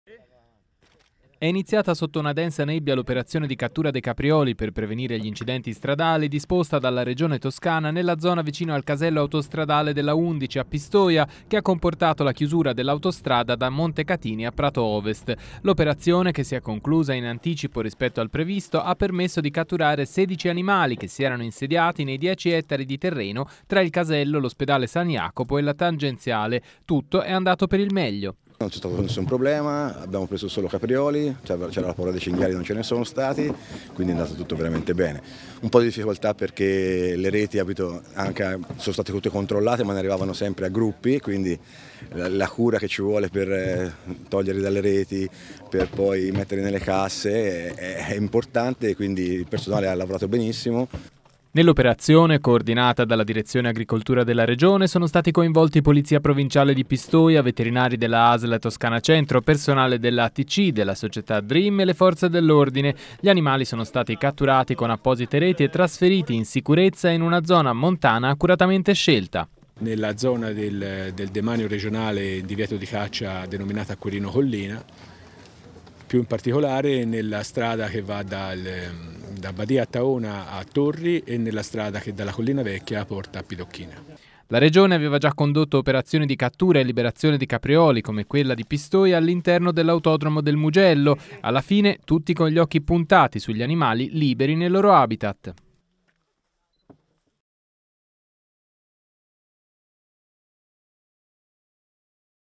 Sedici caprioli liberati nella riserva dell' Acquerino
La Regione ha già condotto altre operazioni di questo tipo, l'ultima nel maggio 2019 all'interno dell'Autodromo del Mugello. La notizia